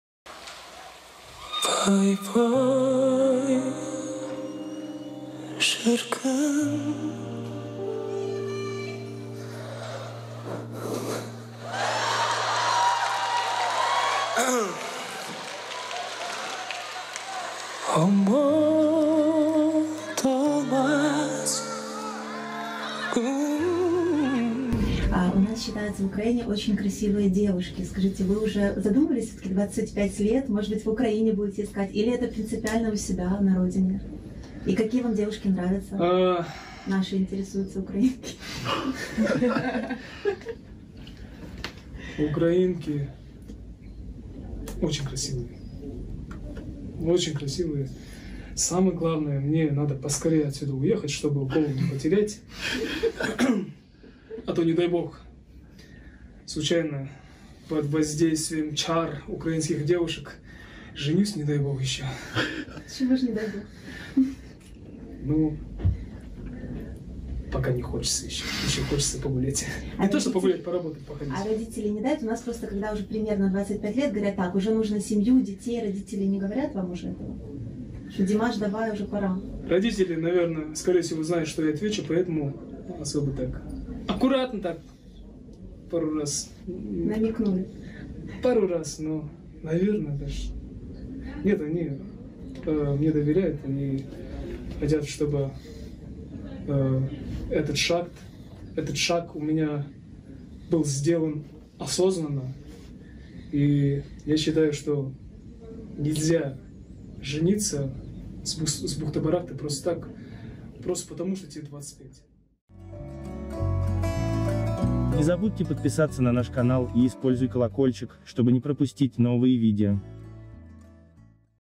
📣 Как украинские девушки смутили Димаша How Ukrainian girls embarrassed Dimash ✯SUB✯
Это было на концерте в Киеве в 2020 году.  Когда из зала вырвался отчаянный крик девушки "любимый", и крик этот слышен был на весь зал. Это и вывело Димаша из образа, а поклонники получили ещё один любимый момент из концерта. Было здорово!
Такие милые и приятные сюрпризы Димаш часто преподносит поскольку он парень простой, милый, добрый.
Как удается Димашу оставаться таким скромным при огромной славе и любви к нему?  Удивительный человек во всем!
It was at a concert in Kiev in 2020. When the desperate cry of the girl "beloved" burst out of the hall, and this scream was heard throughout the hall. This took Dimash out of character, and the fans got another favorite moment from the concert. It was great! Such nice and pleasant surprises Dimash often presents because he is a simple, sweet, kind guy. How does Dimash manage to remain so modest with great fame and love for him? An amazing person in everything!